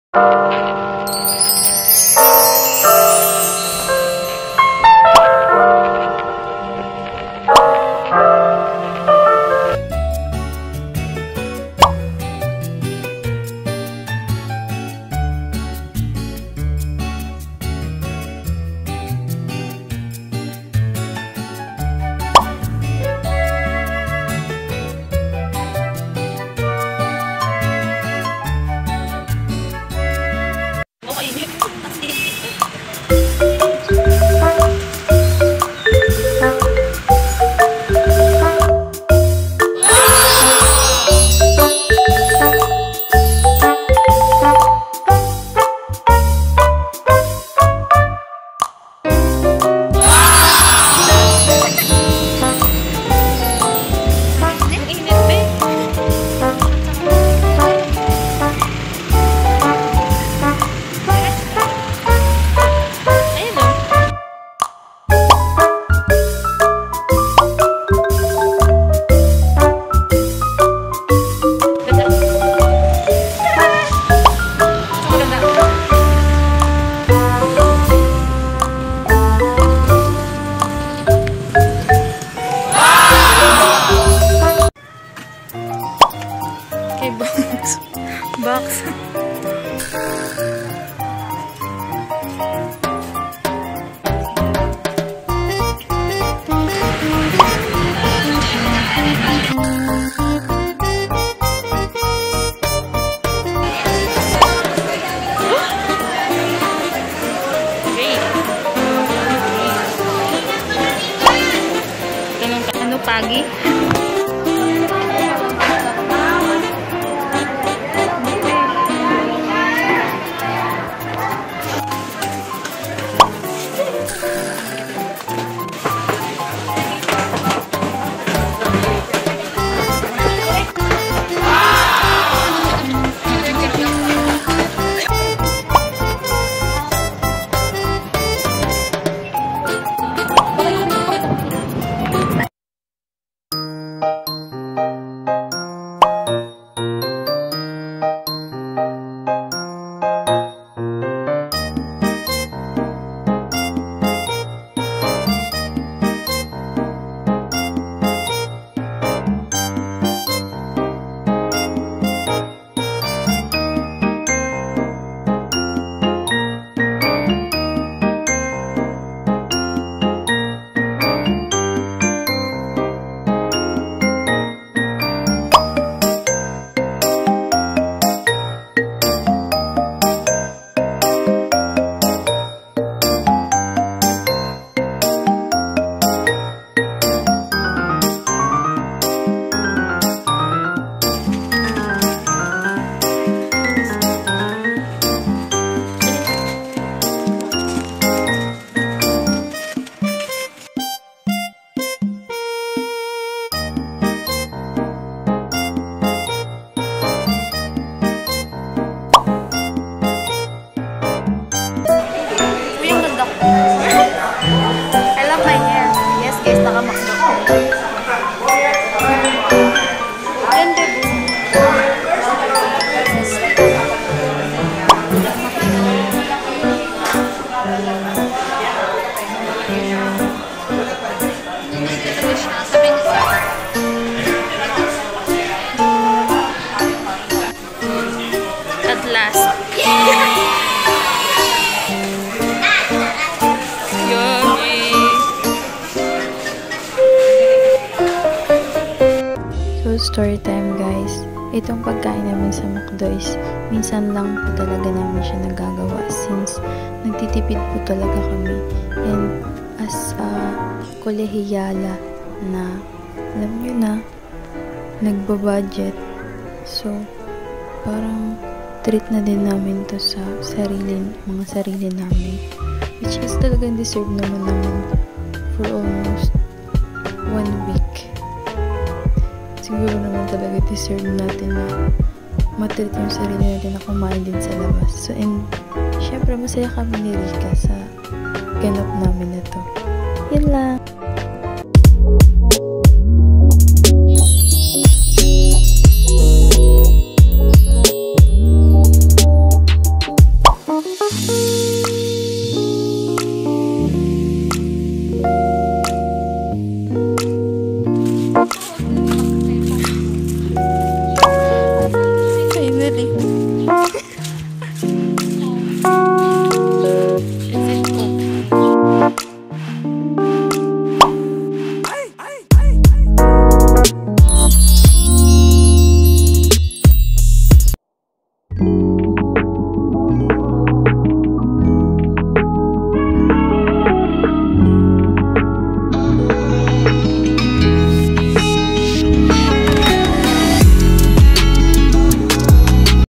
Boarders Life (Kolehiyala Sunday Ganap)
sharing to you all my life every sunday as a kolehiyala na boarder 🤗

hope you enjoy this short vlog!